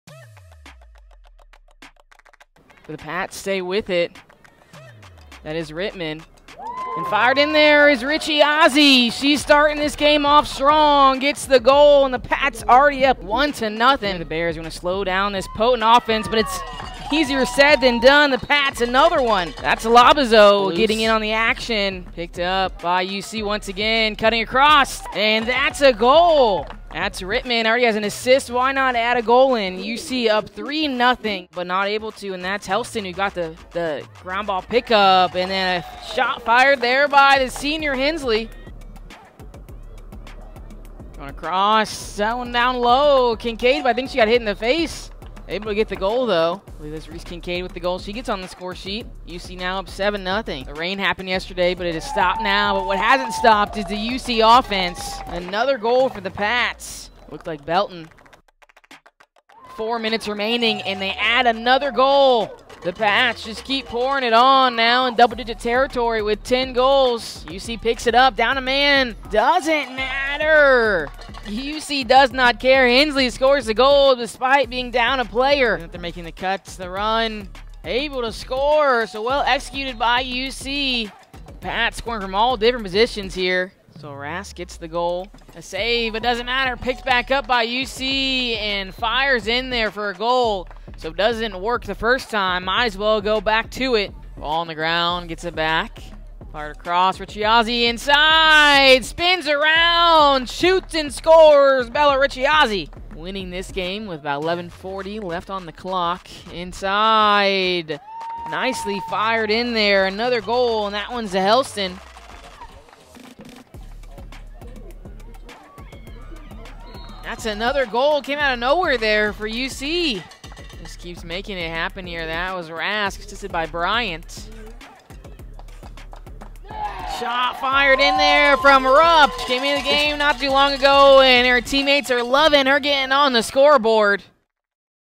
University of the Cumberlands - Women's Lacrosse vs. Truett McConnell University 4-1-2023
#5 University of the Cumberlands Patriots Women's Lacrosse defeats the Bears of Truett McConnell University (W 26-1) in Williamsburg, KY, on April 1st, 2023.